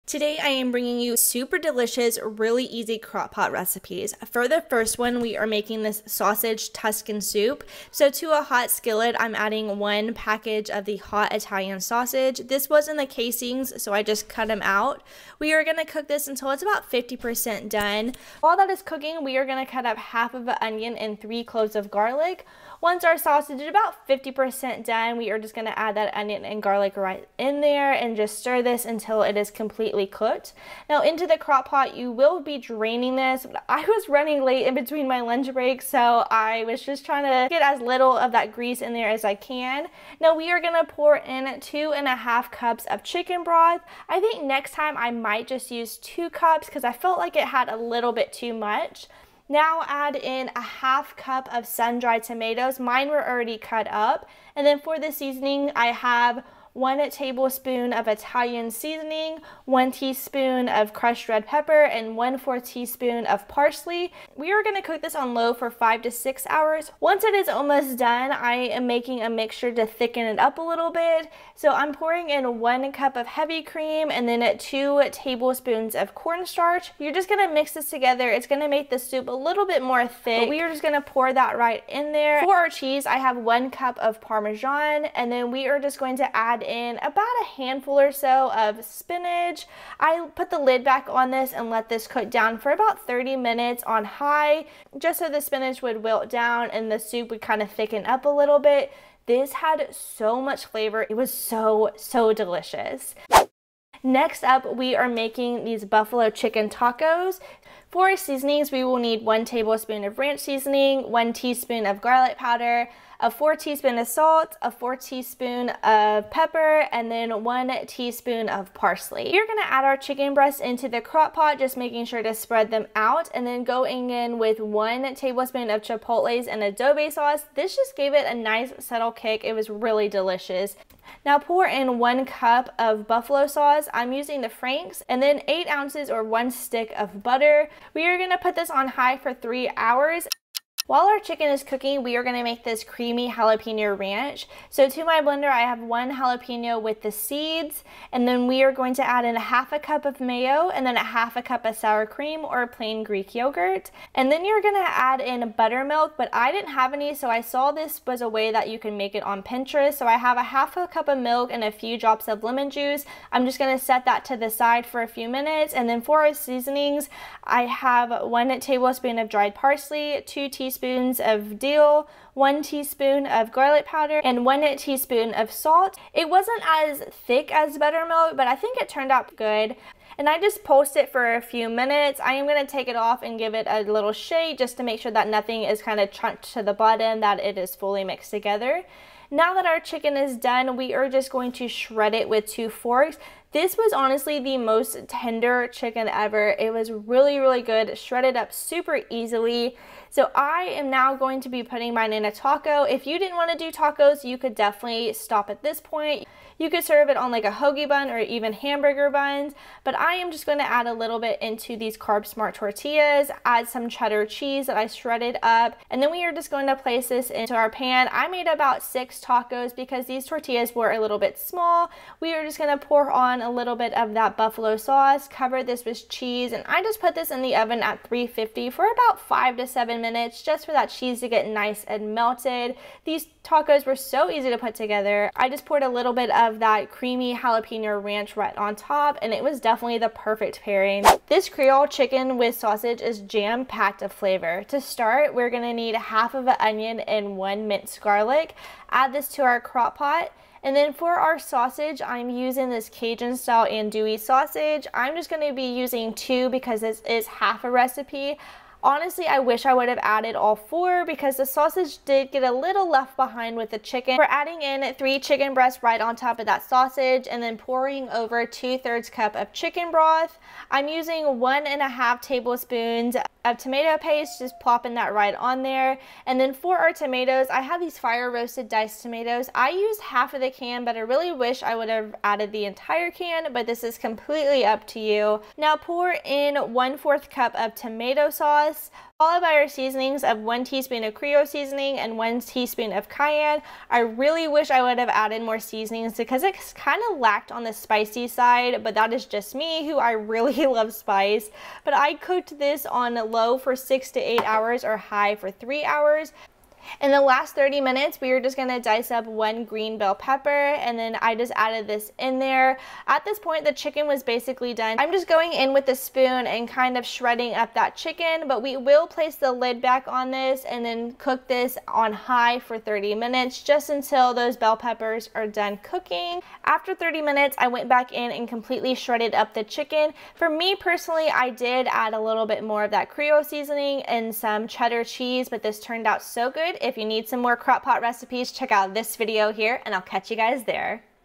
3 New Tasty Crockpot Dinners | Quick Slow Cooker Recipes
EASIEST Slow Cooker DInners | Quick & Tasty Slow Cooker Recipes

Welcome to my cooking channel. Today we are cooking up 3 new crockpot dinner ideas. These meals are so easy to put together. ! I hope you find a some inspriation!

0:00 Welcome *Today's Recipes
0:06 Sausage Tuscan Soup
1:42 Chicken Buffalo Tacos
4:00 Creole Chicken & Sausage

LINKS & DISCOUNTS
Get 2000 Bonus points with FETCH rewards: QNE42Join Ibotta: ashgybl